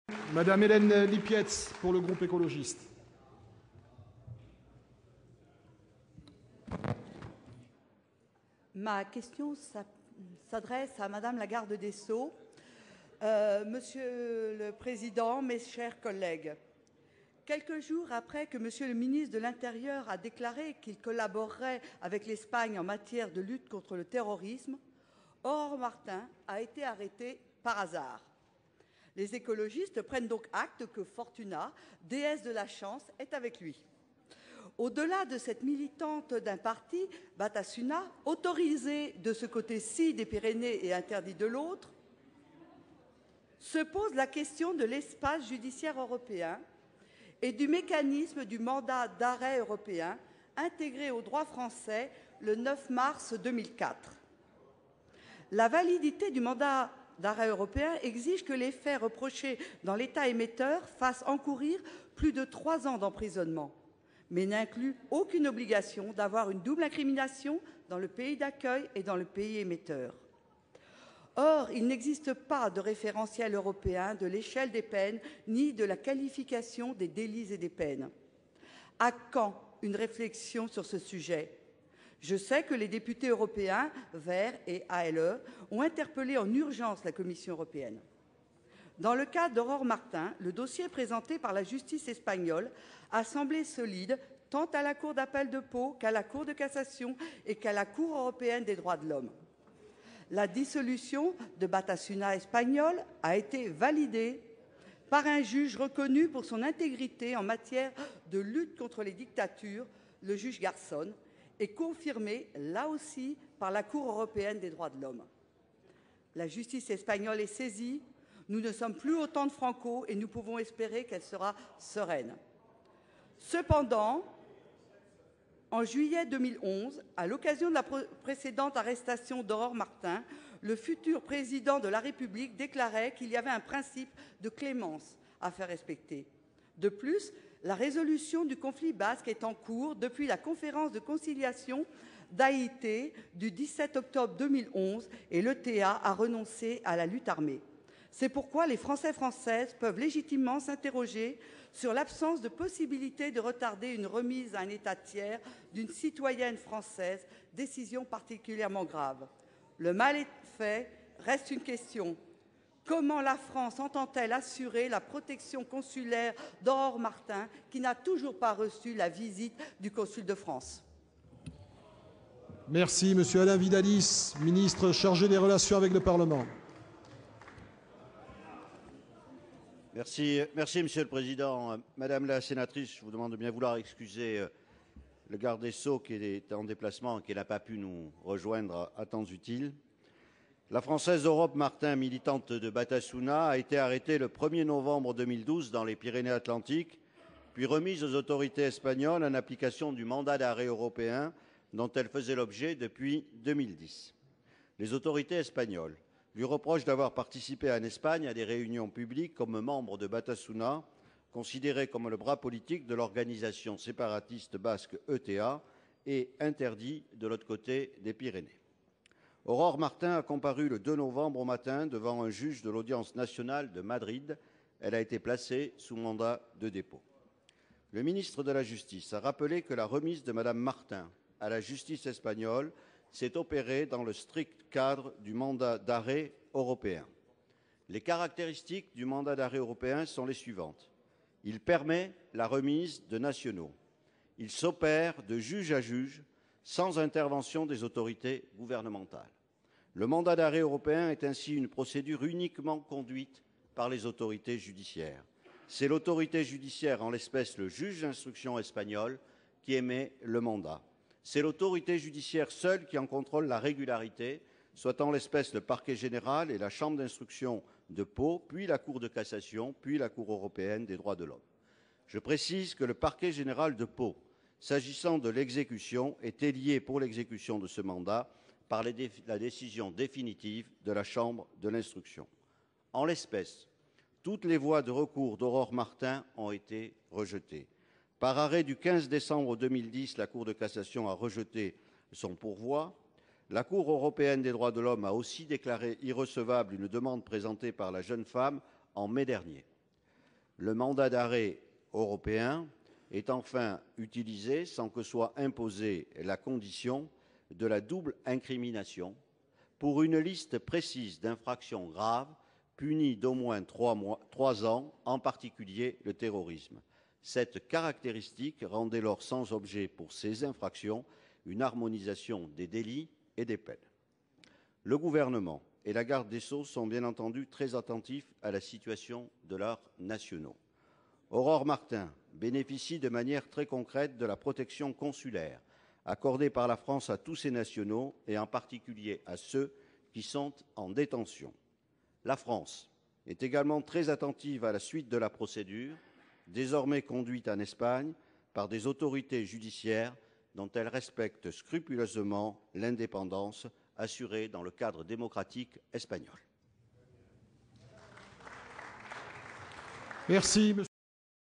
Question d'actualité au gouvernement sur Aurore Martin
Question d'actualité au gouvernement posée par Hélène Lipietz sur l'arrestation et l'extradition de Aurore Martin. Fonctionnement du Mandat d'arrêt européen et protection consulaire de la France pour ses ressortissants!